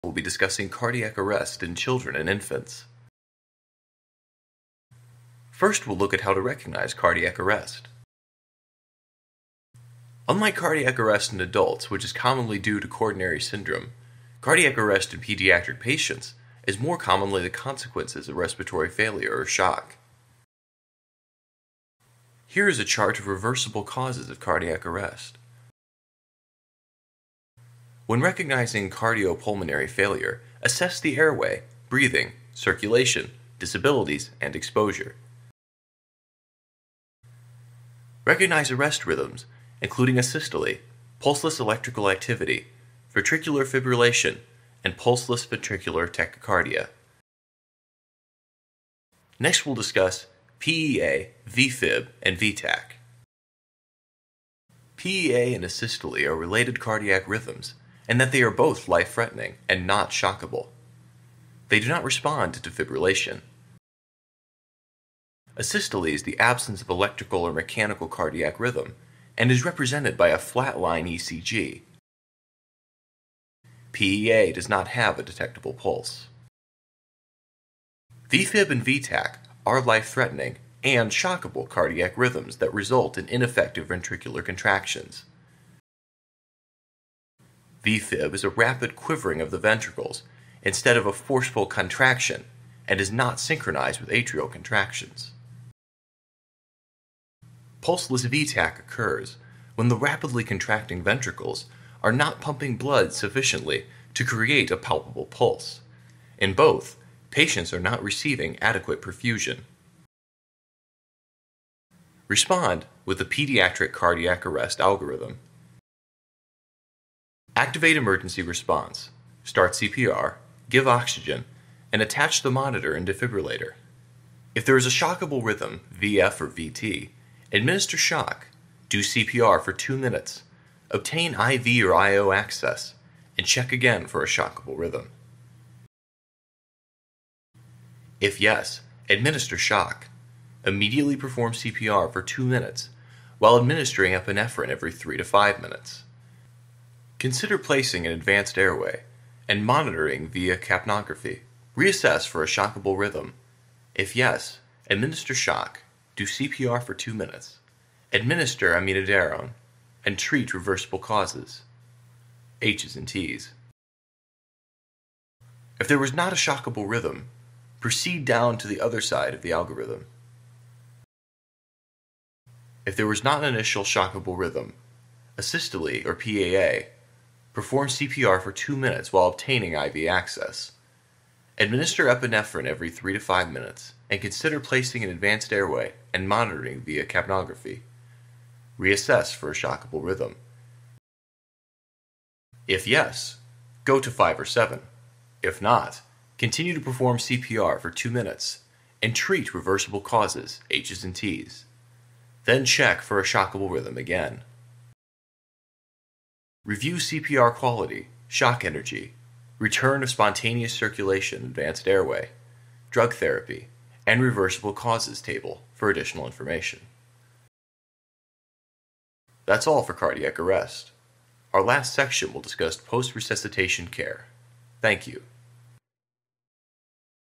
19. PALS - Cardiac Arrest
In this PALS lesson, we will be discussing cardiac arrest in children and infants. You will learn how to recognize the reversible causes of cardiac arrest, cardiopulmonary failure, and arrest rhythms. You will also learn how to recognize and differentiate the different types of abnormal electrical activity of the heart including pulseless electrical activity (PEA), asystole, ventricular fibrillation (VFib), and pulseless ventricular tachycardia (VTach). In order to best comprehend how to respond to cardiac arrest, we have provided you with the algorithm from the manual that we suggest you take the time to thoroughly study.